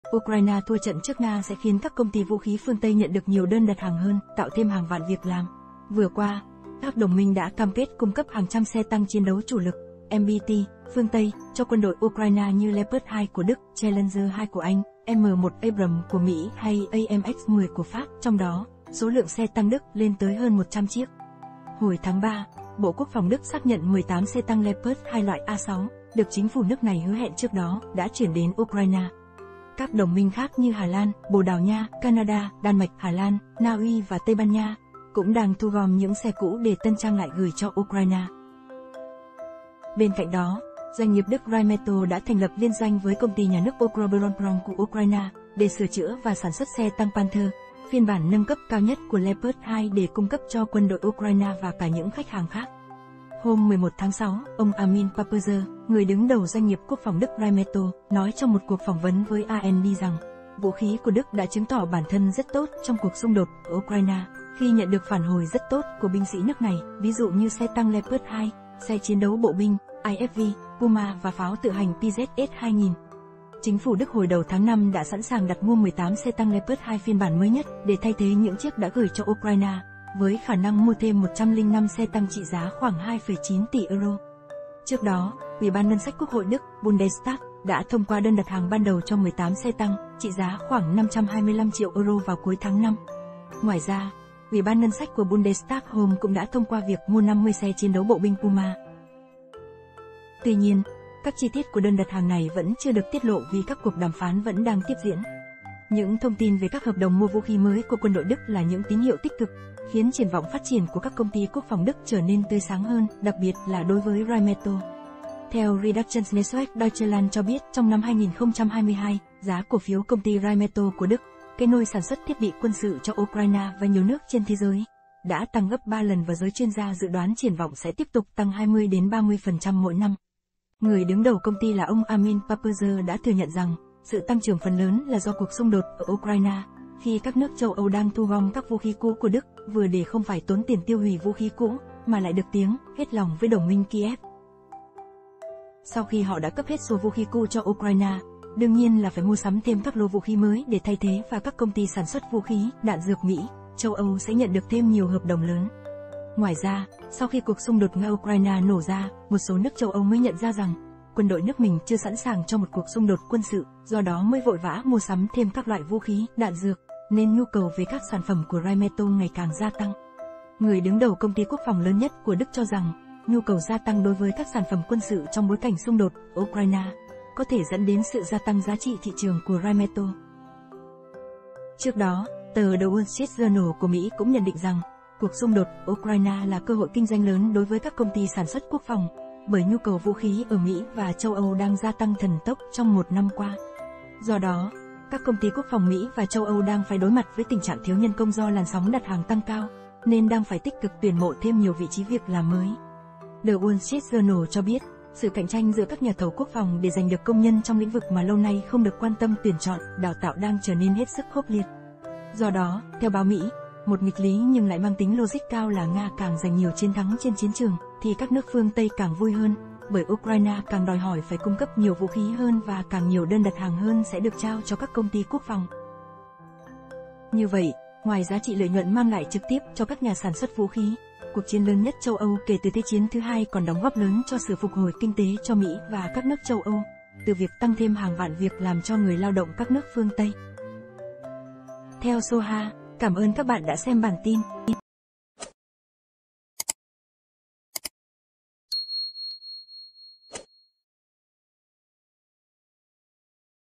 NGA CÀNG THẮNG THÌ CÁC CÔNG TY VŨ KHÍ PHƯƠNG TÂY CÀNG MỪNG RỠ
Ukraine thua trận trước Nga sẽ khiến các công ty vũ khí phương Tây nhận được nhiều đơn đặt hàng hơn, tạo thêm hàng vạn việc làm. Vừa qua, các đồng minh đã cam kết cung cấp hàng trăm xe tăng chiến đấu chủ lực (MBT) phương Tây, cho Quân đội Ukraine như Leopard 2 của Đức, Challenger 2 của Anh, M1 Abrams của Mỹ hay AMX-10 của Pháp, trong đó, số lượng xe tăng Đức lên tới hơn 100 chiếc.